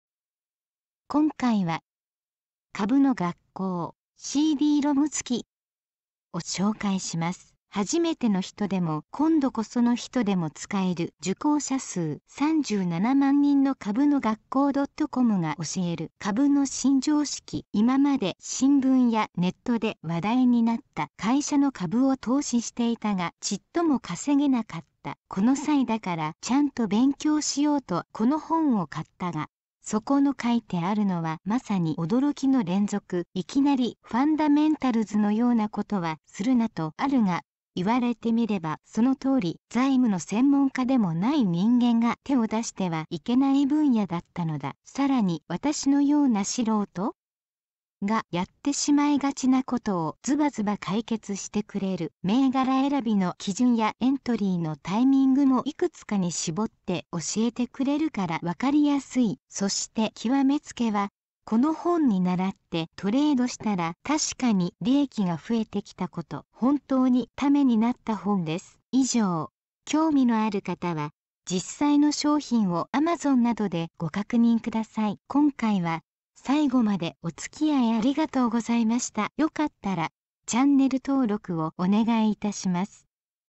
【紹介】株の学校CD ROM付 （窪田 剛,柴田 博人）
今回は、　株の学校(CD-ROM付)　を紹介します。

“はじめて”の人でも“こんどこそ”の人でも使える受講者数37万人の株の学校ドットコムが教える株の新常識。

いままで新聞やネットで話題になった会社の株を投資していたがちっとも稼げなかった。この際だからちゃんと勉強しようとこの本を買ったが、そこの書いてあるのはまさに驚きの連続。いきなりファンダメンタルズのようなことはするなとあるが、言われてみればその通り。財務の専門家でもない人間が手を出してはいけない分野だったのだ。さらに私のような素人？がやってしまいがちなことをズバズバ解決してくれる。銘柄選びの基準やエントリーのタイミングもいくつかに絞って教えてくれるからわかりやすい。そして極めつけは、この本に倣ってトレードしたらたしかに利益が増えてきたこと。本当にためになった本です。